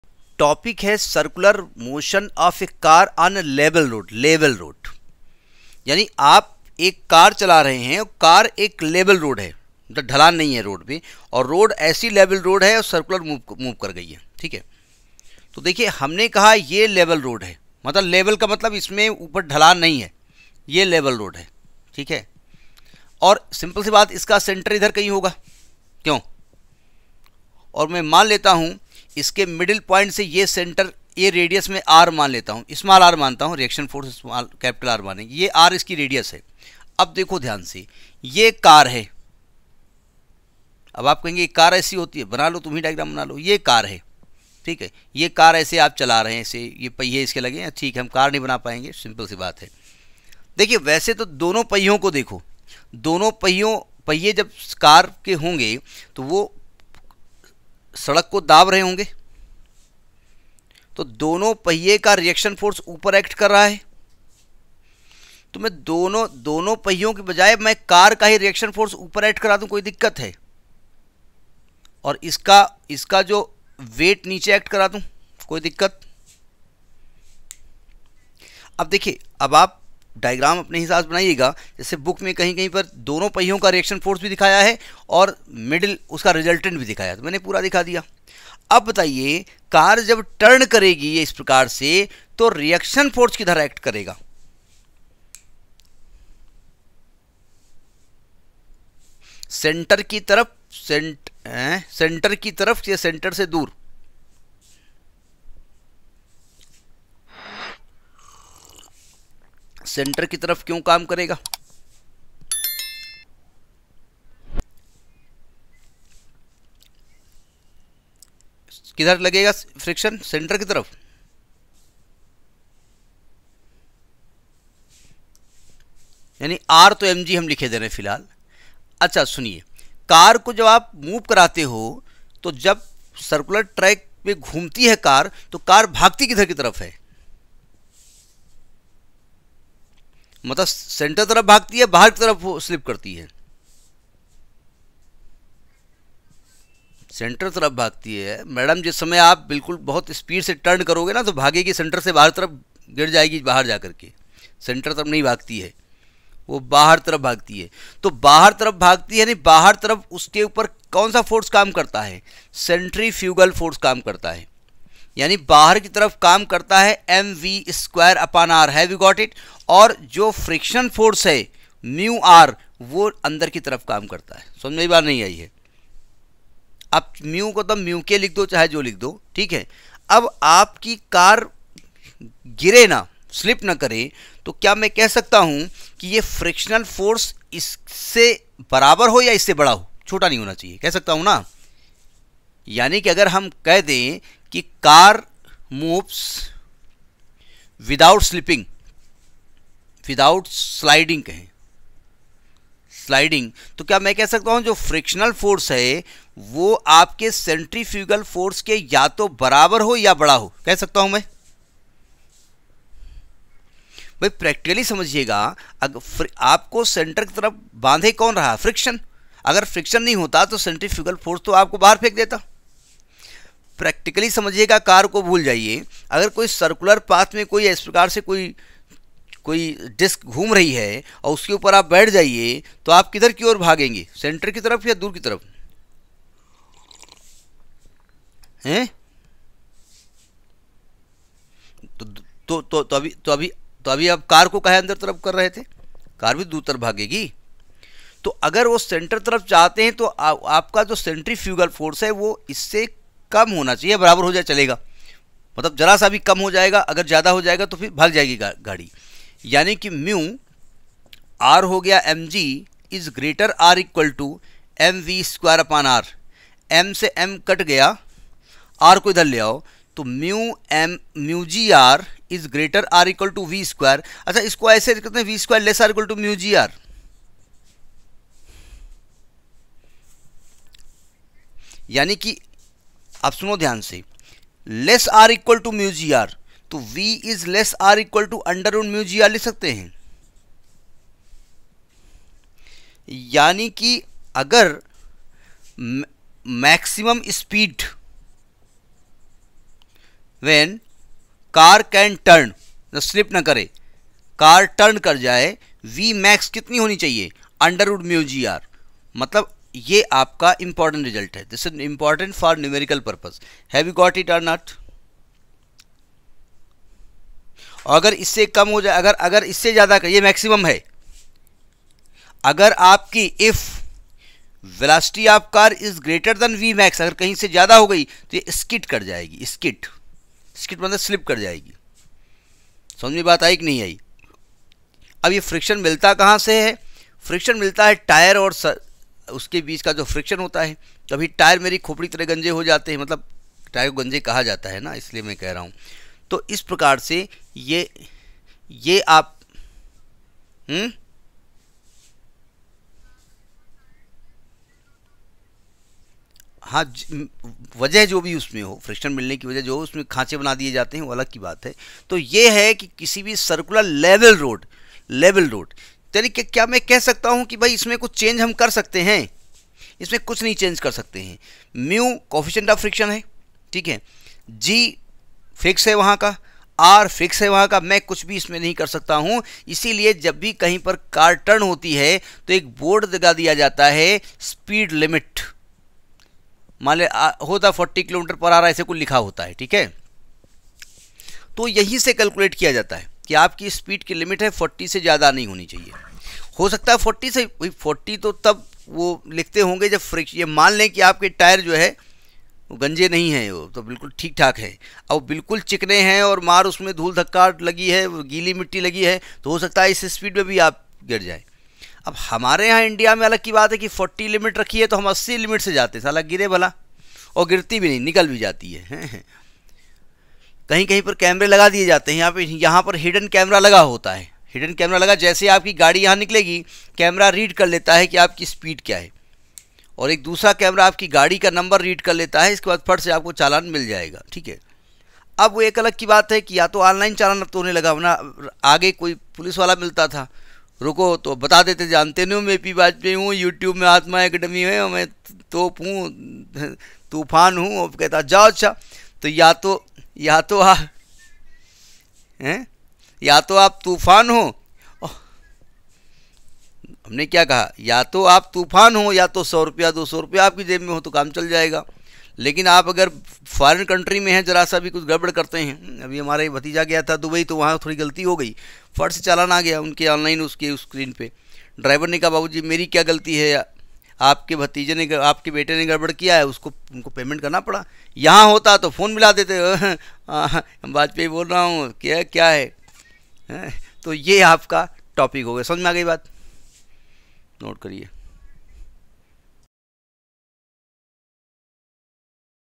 NEET Physics; “ Circular motion of car on a level road “.
NEET Physics; “ Circular motion of car on a level road “.@AtmaAcademy

In this video I have discussed NEET PHYSICS.
In this video I have discussed the Circular motion of a car on Level road.

Language: English mixed Hindi